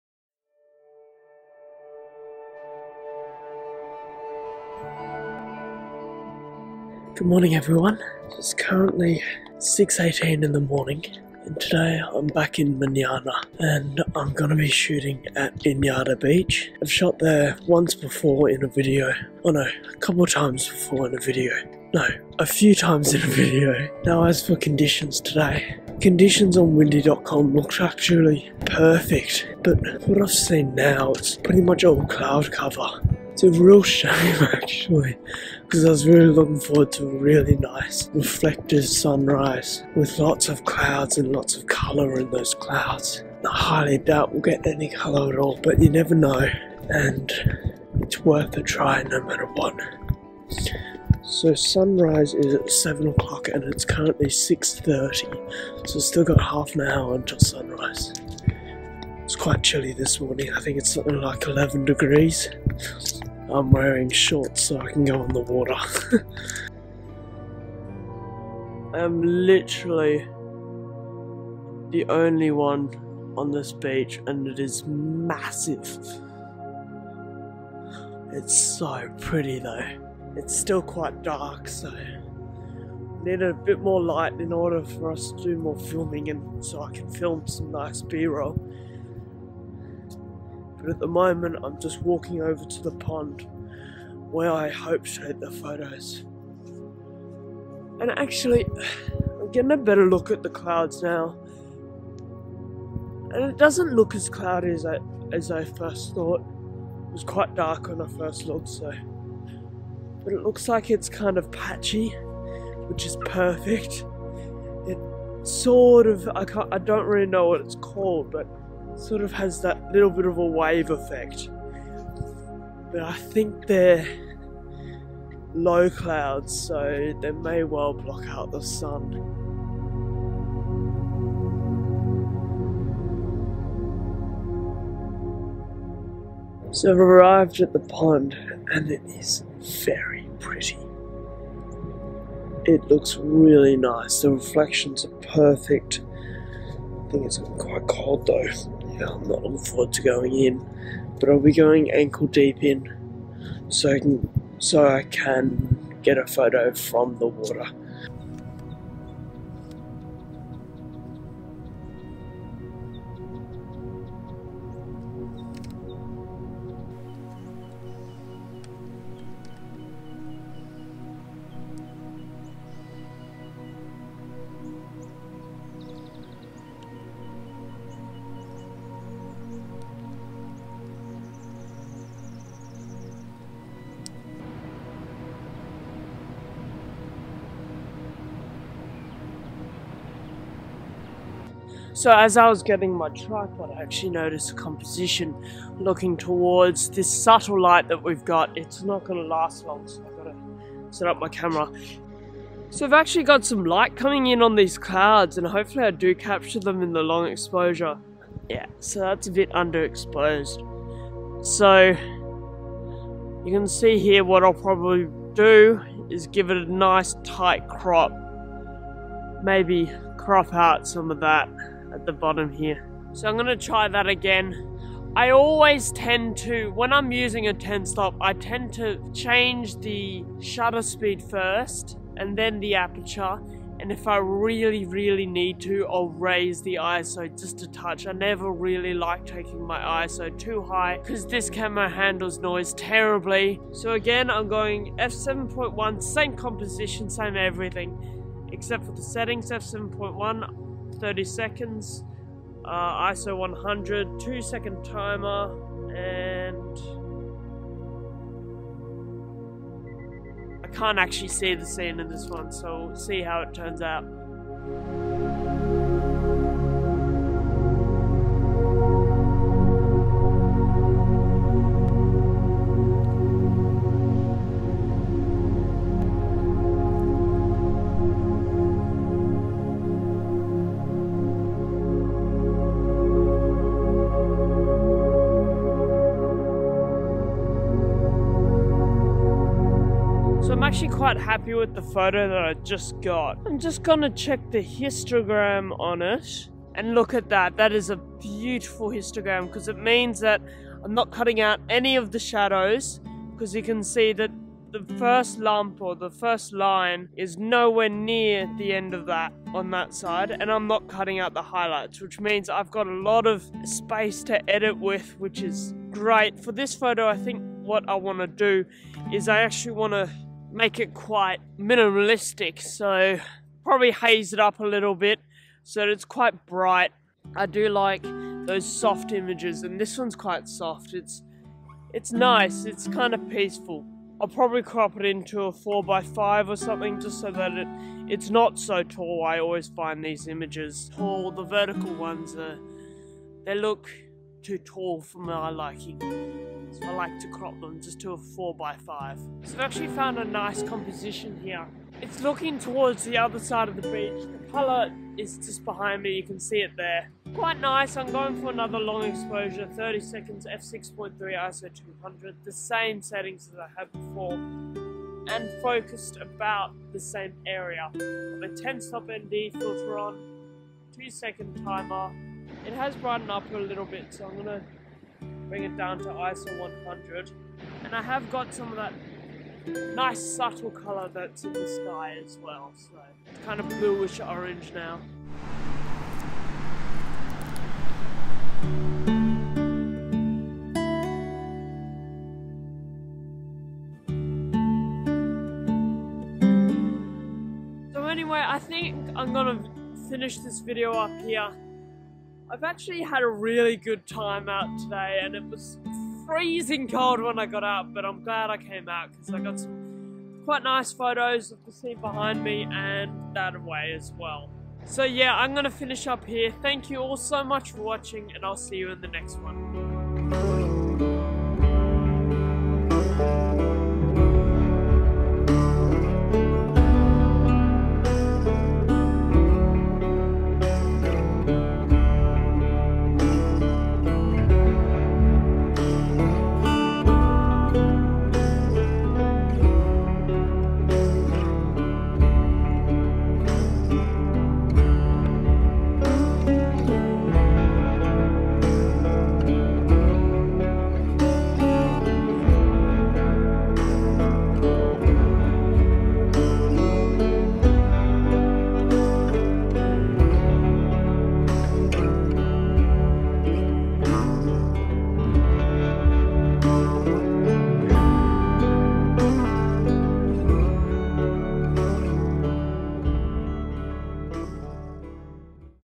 Capturing Emotion at Inyadda Beach - Long Exposure Photography
In this video, I take some moody long exposure photos at sunrise at Inyadda Beach